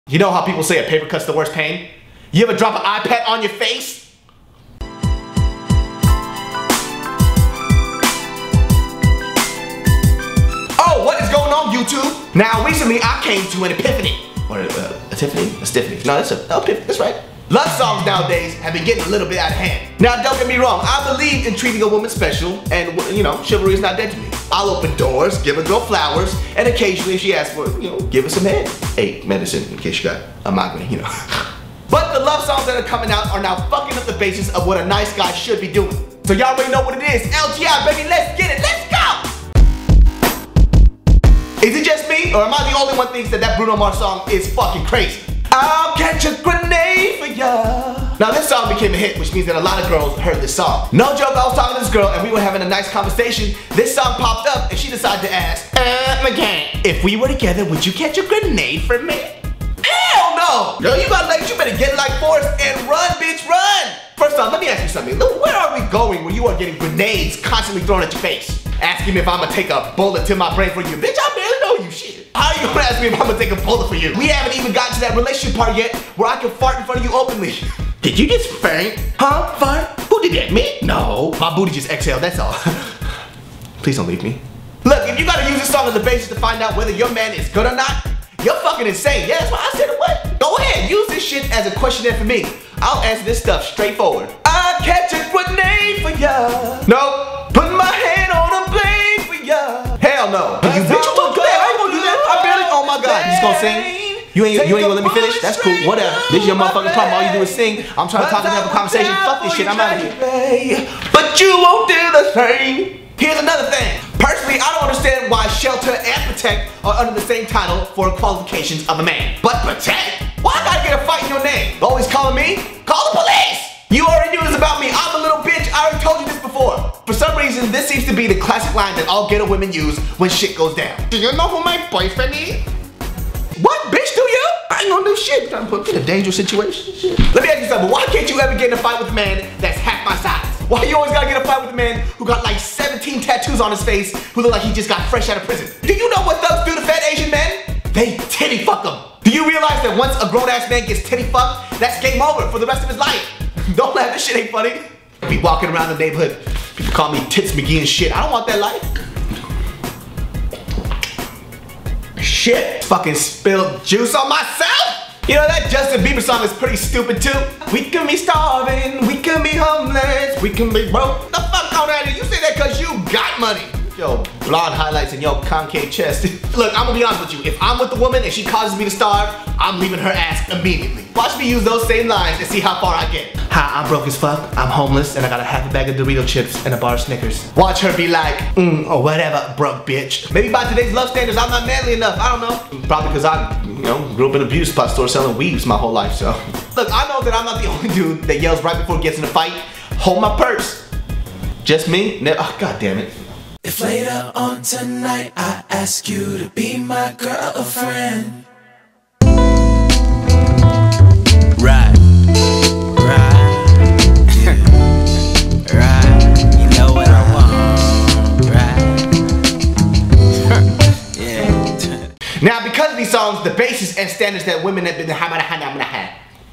VLOG 71: love me like BRUNO
Love music has been getting pretty intense lately. I'm not sure if its because we're running out of things to sing about, but the realism is gone. Now we're just singing crazy shit. LOL

I'm trying to at least bleep the F words but its almost 7 am and i've been editing for 5-6 hours straight after shooting and writing and I'm spent. so fucken deal with it LOL! JK... kinda...